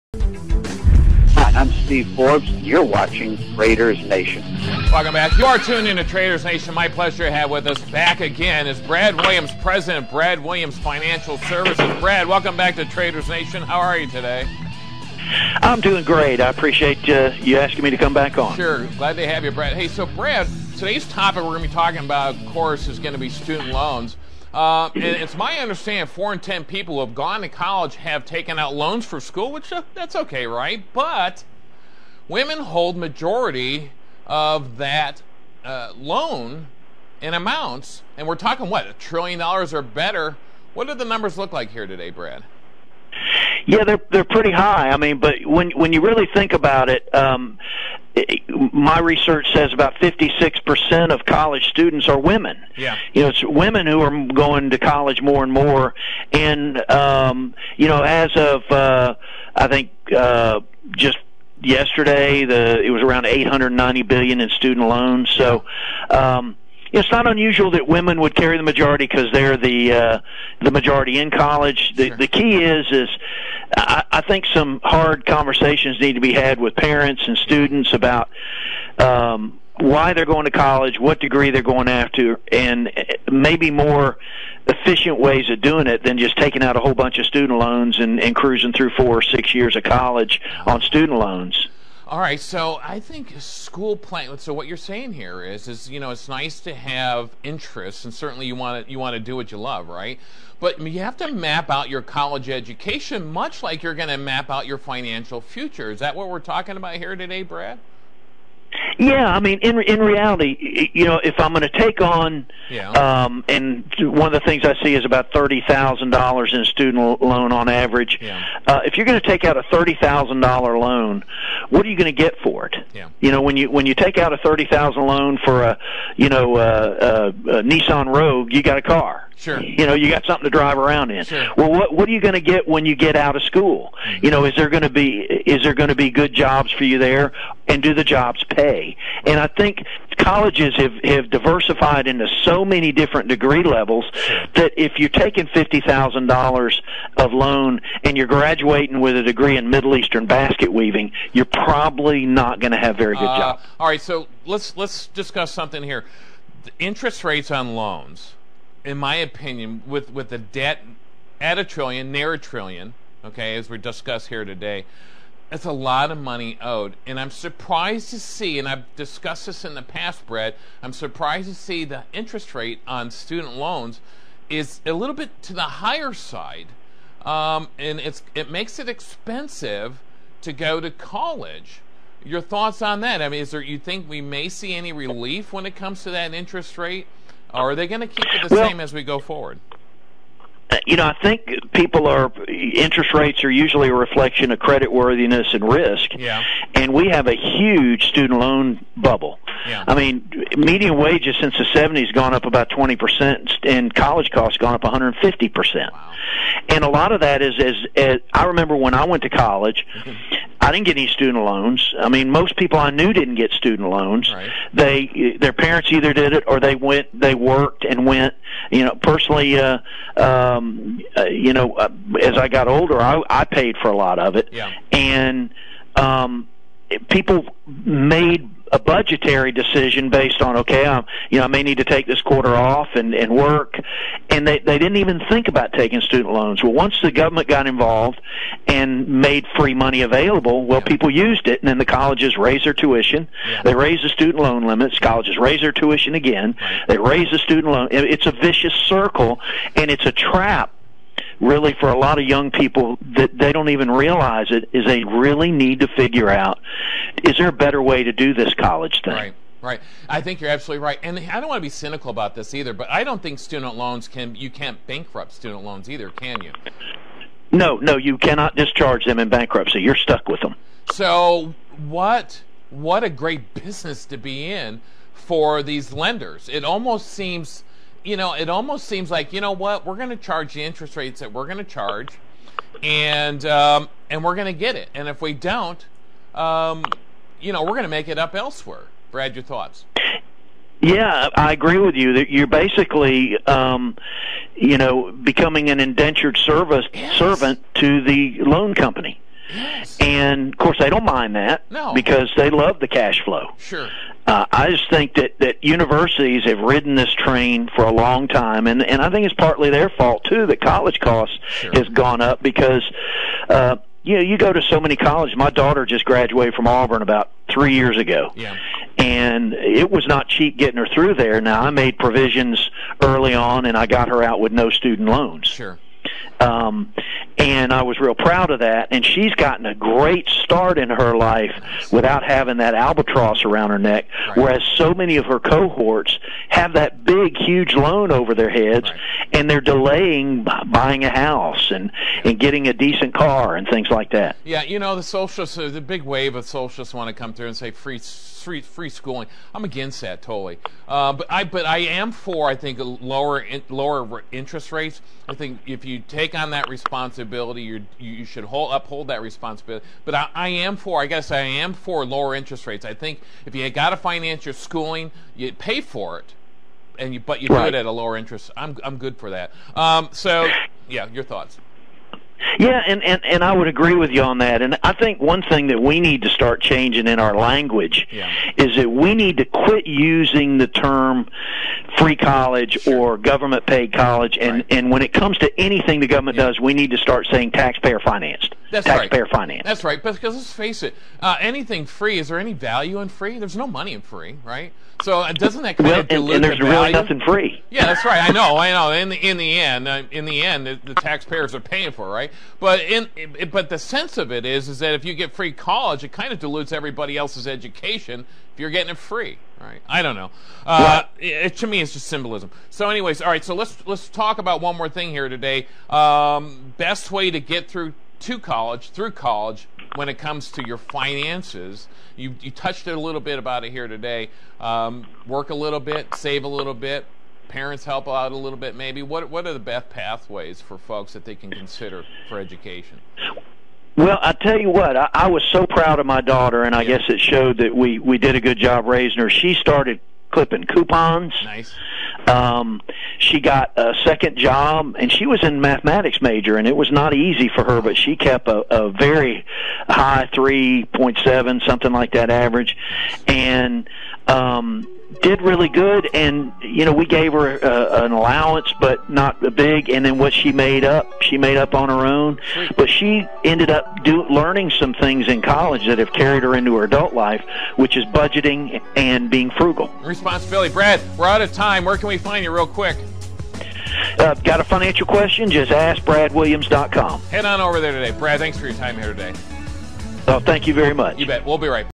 Education: It’s not cheap, as student loan debt nears a trillion dollars
Brad Williams, President of Brad Williams Financial Services discusses with Kurt Schemers on Traders Nation® how student loan debt has skyrocketed to nearly a trillion dollars and that women hold the majority of that debt. Get the numbers of this phenomenon broken down and the reasons why.

Additionally, students and parents need to have a hard conversation to properly map out their education that can carry the student into a career future. Often, thousands of dollars are spent on classes that don’t translate into a lifetime of employment ROI. Further, we touch on student loan interest rates, and the cost of “free” education – is it really free?